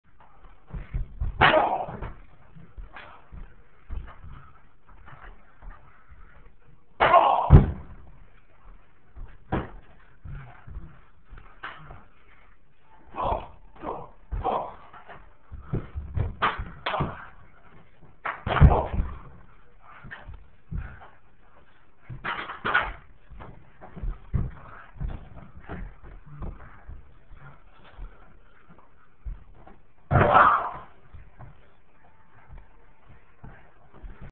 Chair shots and Rock Bottom through the table!
Punch in the face with the chair followed with a chair shot. guillotine countered with a ddt into the chair. finished off with a Rock Bottom through the table!